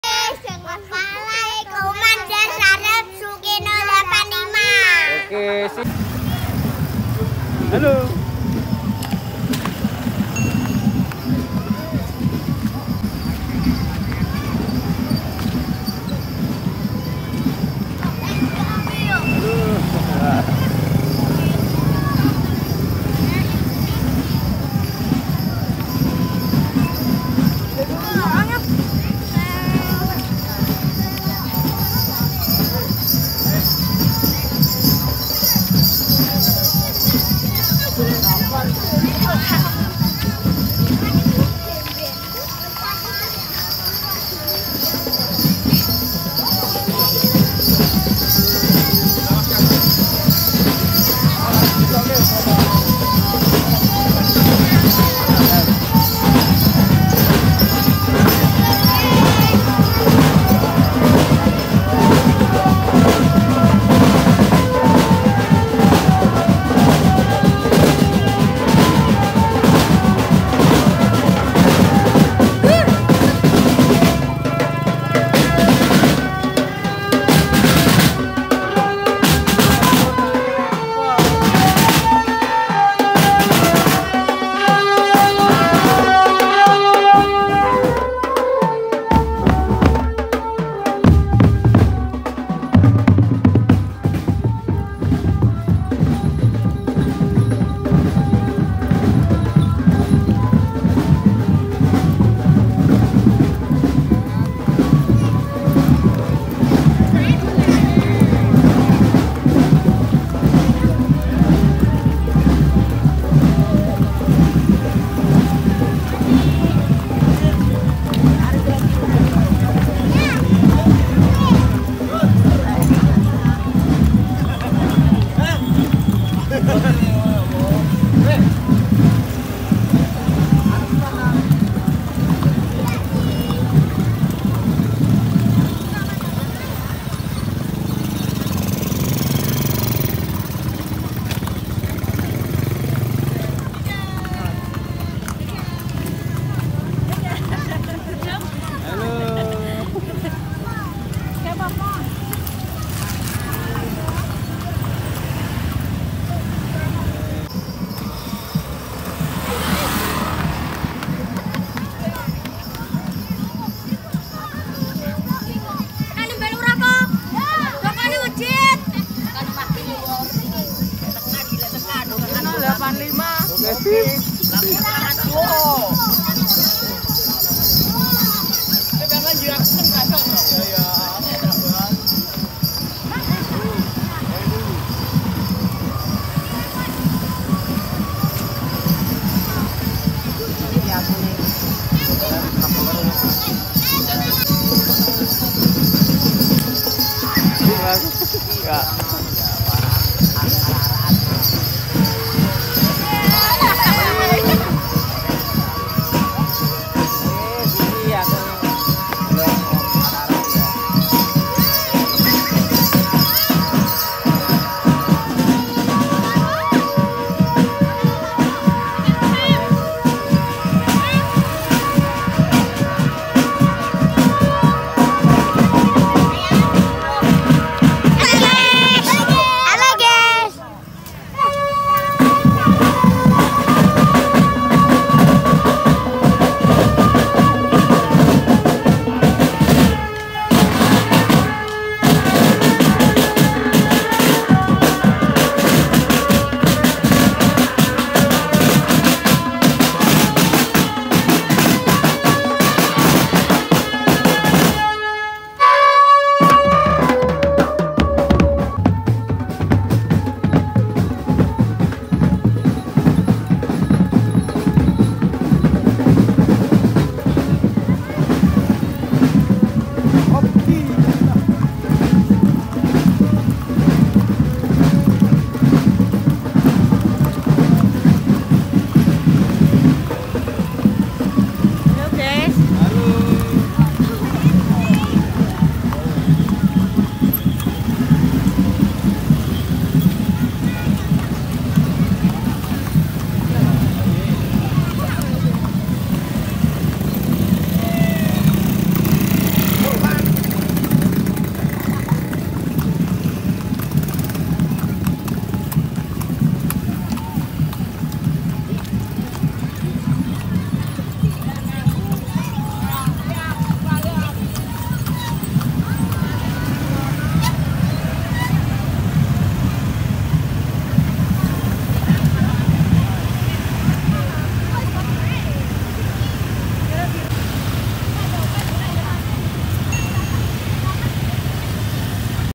KIRAB KHITAN BARONGAN DRUMBAND DEWA-DEWI AL-HUDA DI DUSUN GATAK TLOGOWERU-GUNTUR.
Acara ini dalam rangka walimatul khitan yang dilaksanakan didusun gatak tlogoweru -guntur
Yang di meriahkan seni barong,Drumband Dewa-Dewi al-huda dari desa guntur.

Difollow yah

Facebok : Sukirno Kamandanu
Istagram: sukirno kamandanu